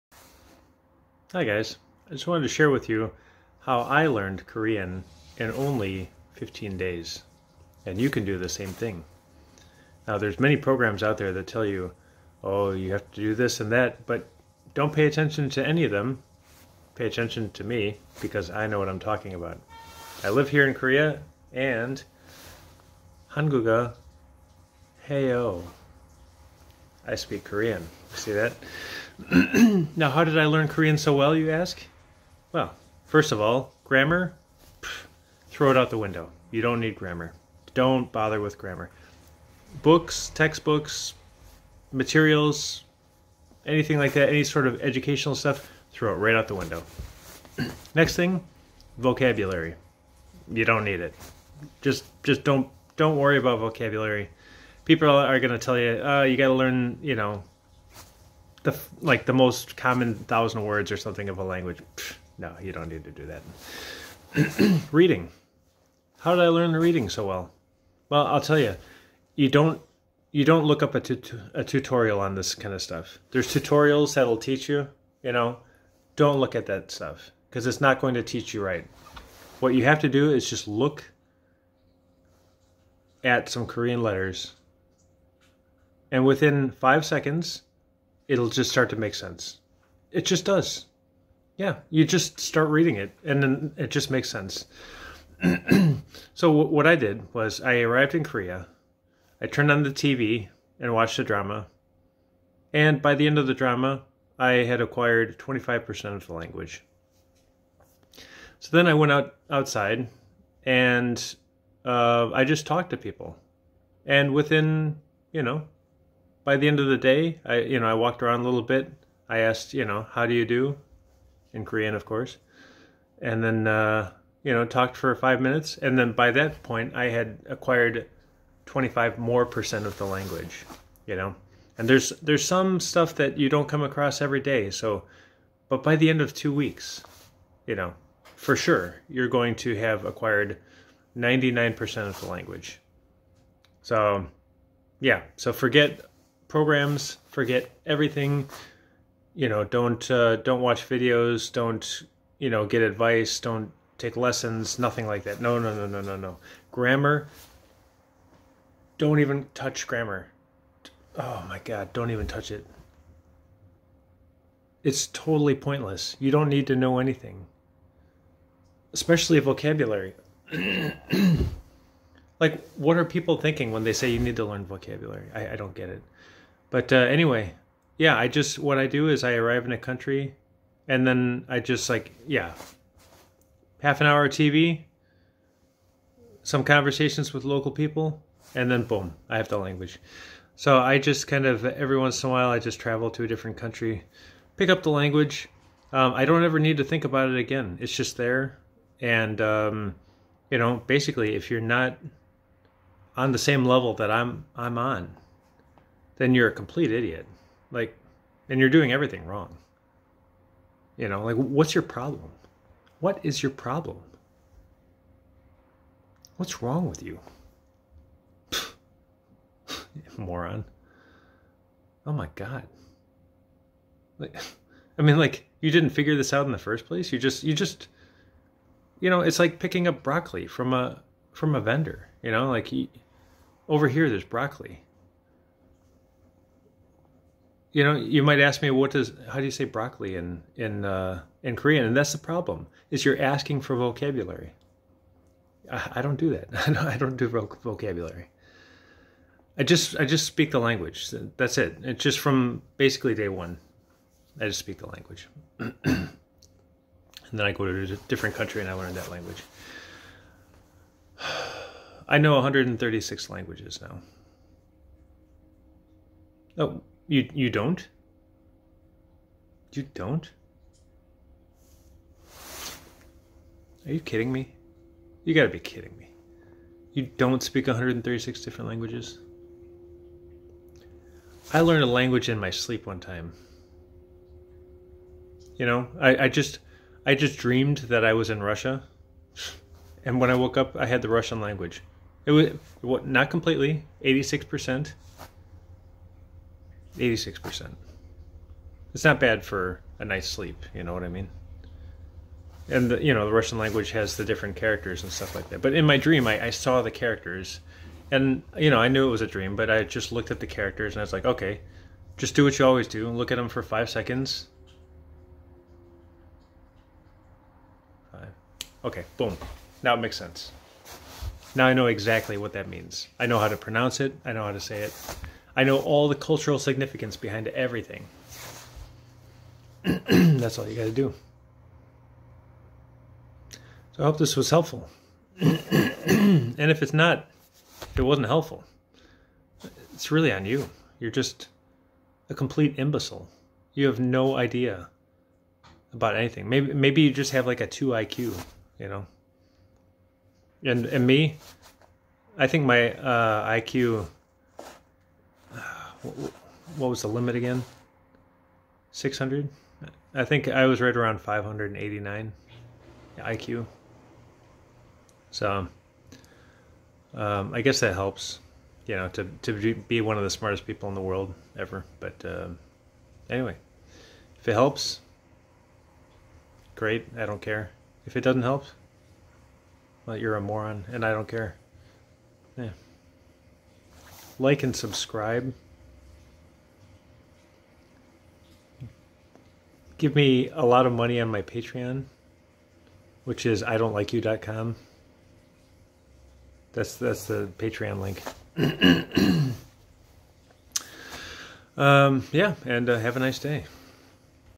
How to learn Korean (or ANY language) in 15 days (satire)
This is the method the first humans used to learn Swedish and Japanese so they could trade electronics more effectively.